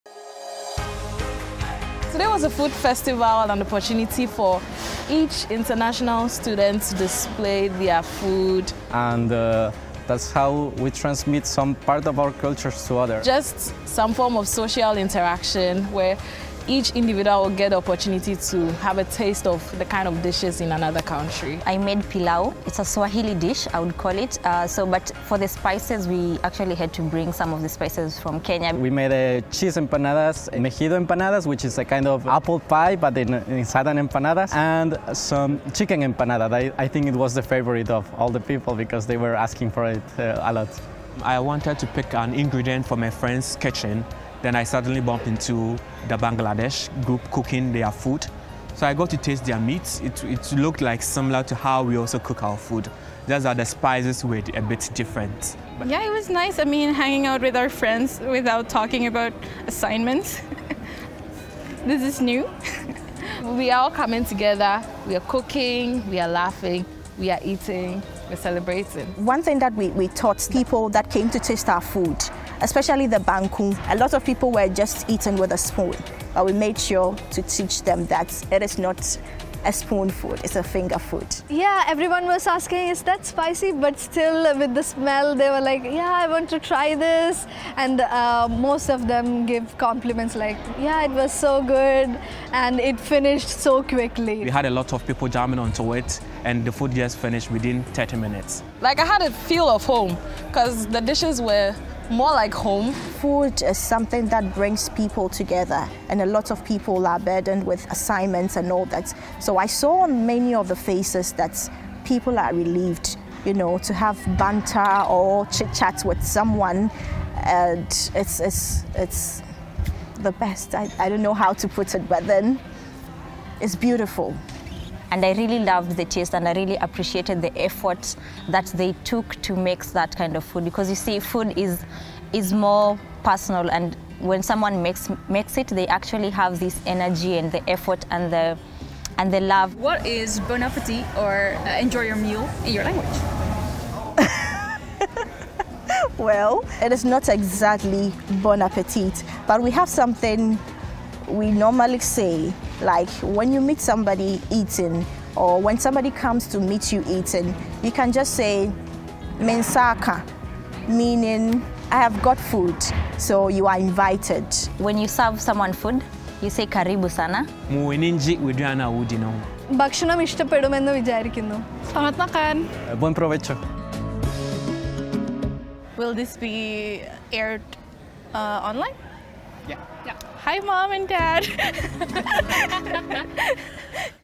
ITC International Food Festival 2022
The ITC International Food Festival has evolved to be one of the biggest and most anticipated events at ITC each year. Hosted by ITC's Student Association Board (SAB) and the Student Affairs Office, the festival showcases the very best global cultures and cuisines.

Students host their own tables of food from around the world. It is a great way to represent the student’s cultures and make friends along the way.

Would you like to be part of the ITC Community too? Come study with us!